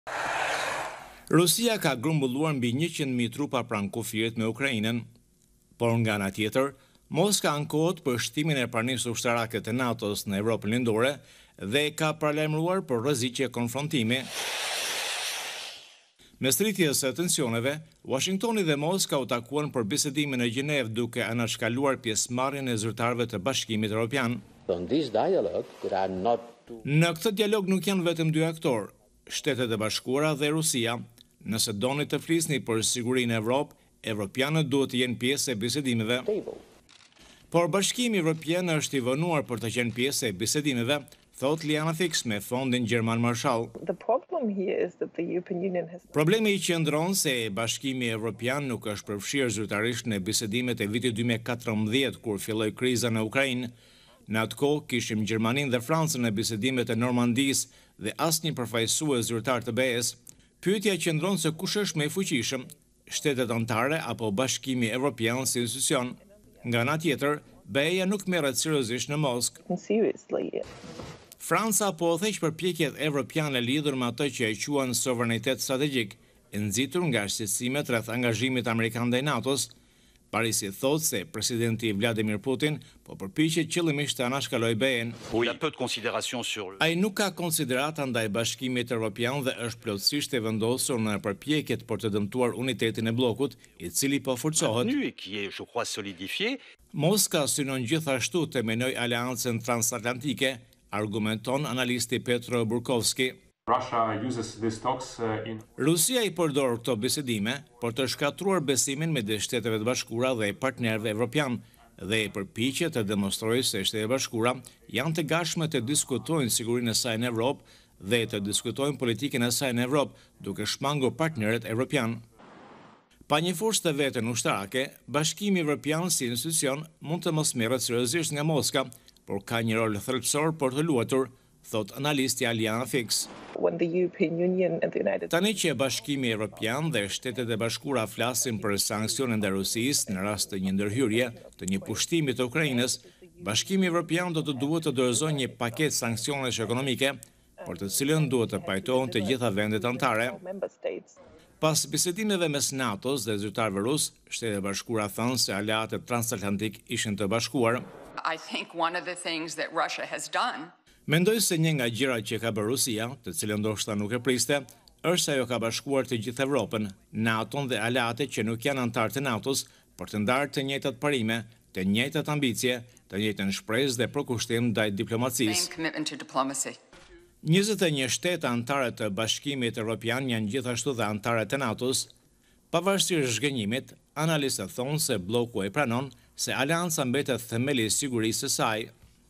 Evropianët ndjehen të përjashtuar nga bisedimet SHBA-Rusi
Ndërsa Shtetet e Bashkuara dhe Rusia zhvilluan bisedime në Gjenevë këtë javë mbi sigurinë e Evropës, në tryezën e bisedimeve mungonte Bashkimi Evropian, duke zhgënjyer zyrtarët në bllokut.